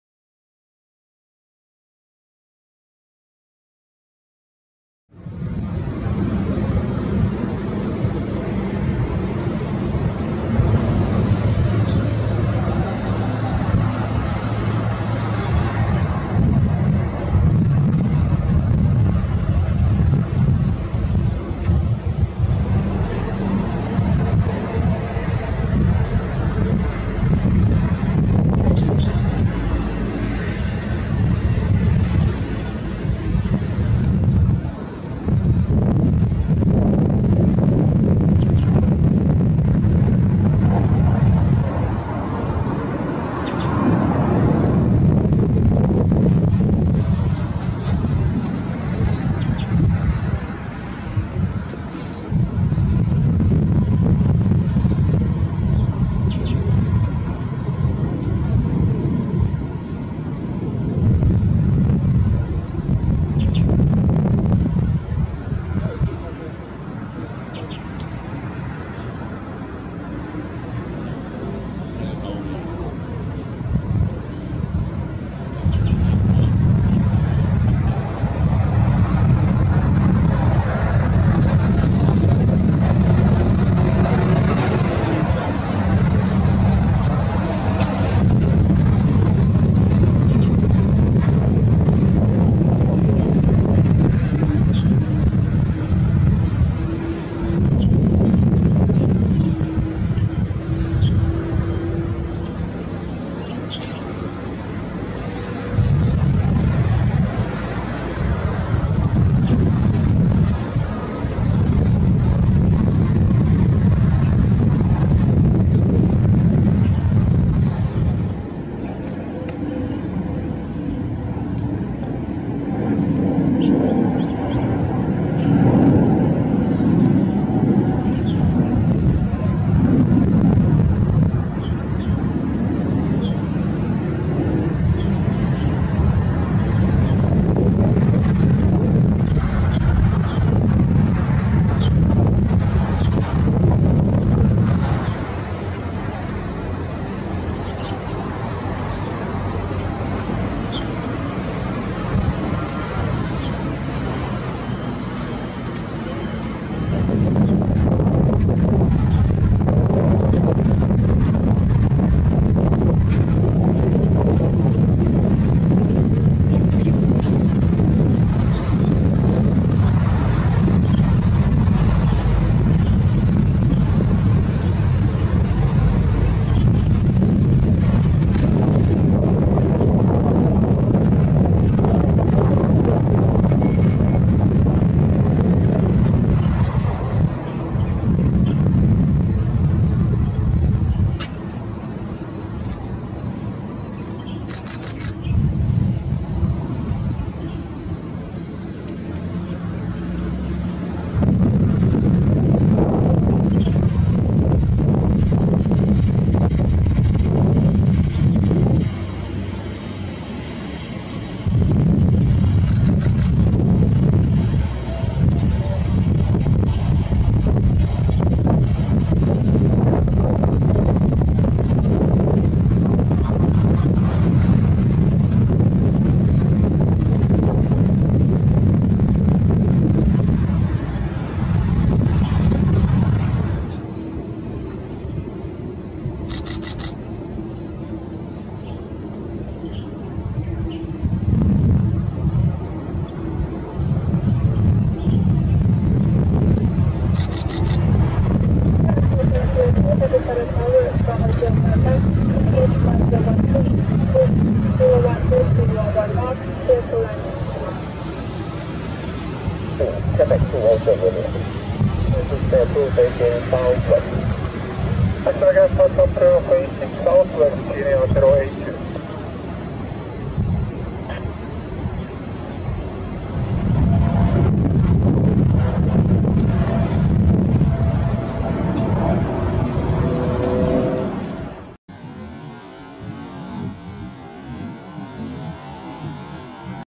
Departure of Boeing 747-8 of Korean Air@臺灣桃園國際機場
HL7633 Delivered November 2015 with a configuration of F6C48Y314. Taiwan Taoyuan International Airport.